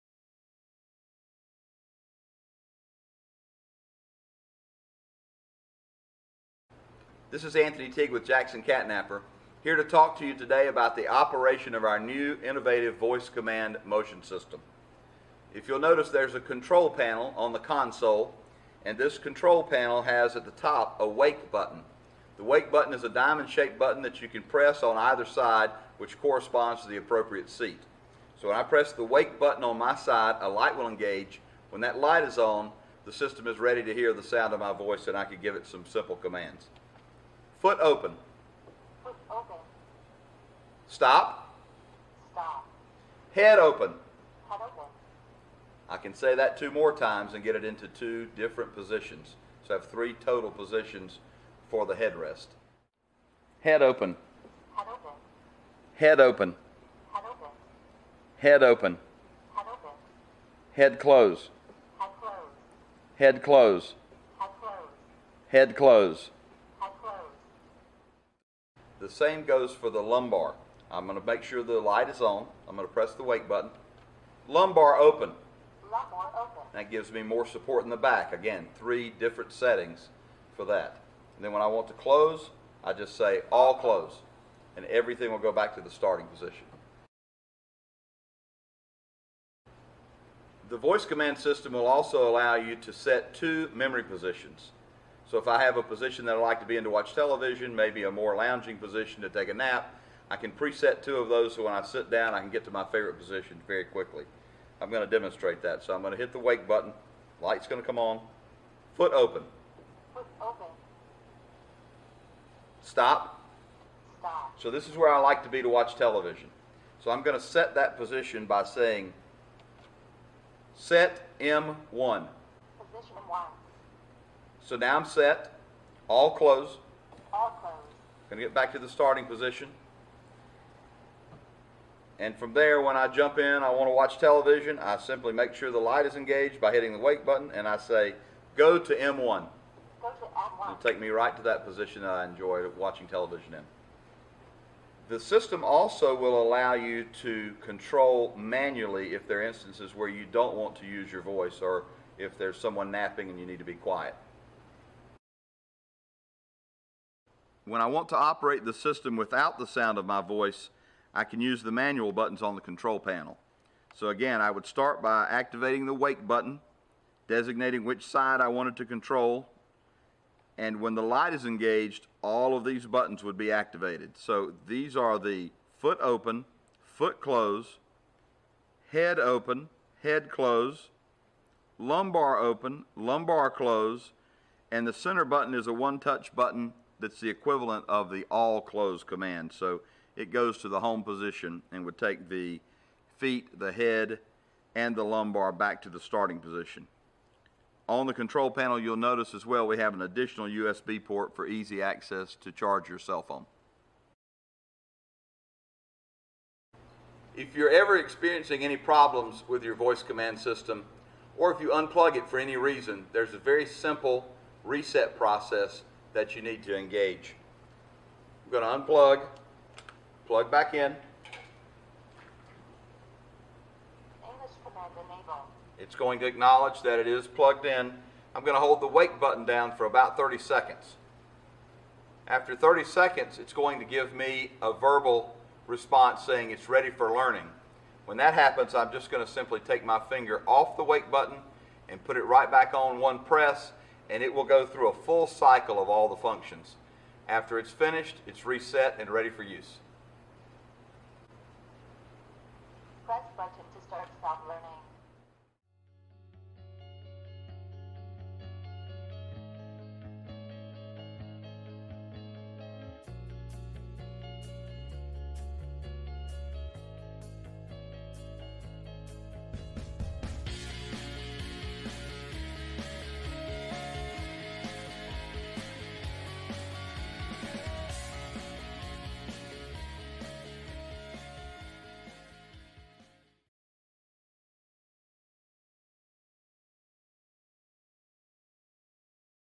Demonstration of all Catnapper Voice Commands (Guide)
This video will guide you on all of the features of the voice activation reclining mechanic of our catnapper voice command line.

BACKSTORE